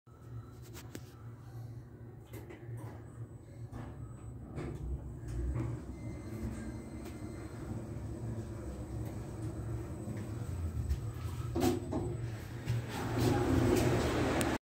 Westfield Riccarton - Schindler E-Type Panoramic Elevator
The budget line of installations offered by Schindler, and can be found in Thailand, Malaysia and New Zealand. The fixtures resemble something closer to a generic elevator manufacturer.

Technical specifications :
Floor served : 1, 2
Capacity: 13 persons/1000kg
Manufacturer : Schindler
Year installed: 2005
History: Likely modernized